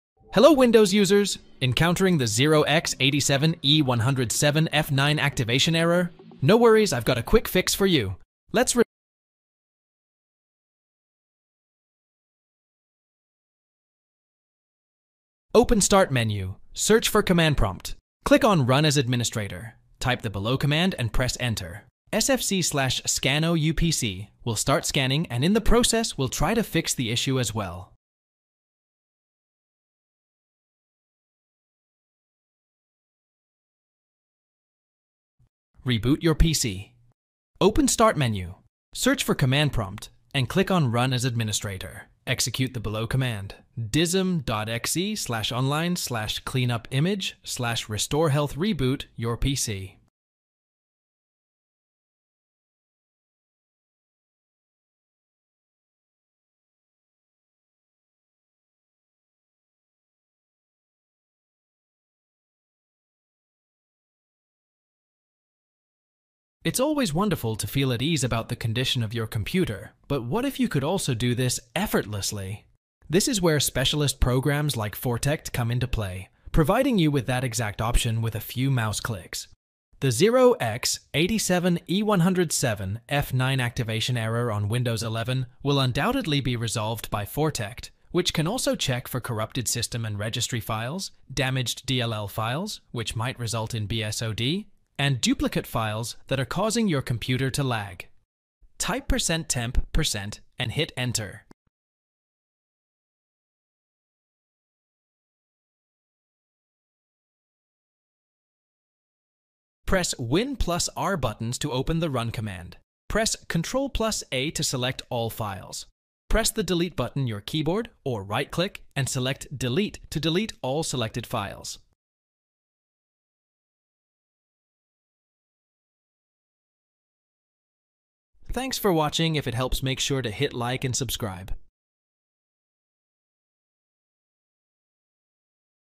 FIX Windows Activation error 0x87e10bc6 on Windows 11
Encountering Windows activation error 0x87e10bc6 on Windows 11? Discover expert insights on troubleshooting and resolving this common issue. Uncover the reasons behind the activation error and learn essential tips for fixing it effectively. Follow our step-by-step guide to ensure seamless activation and optimal performance on your Windows 11 system.

00:00 Intro
00:10 Run SFC Scan
00:29 Run DISM Command
01:00 Use a specialized software
01:44 Clear temporary folder
02:10 Outro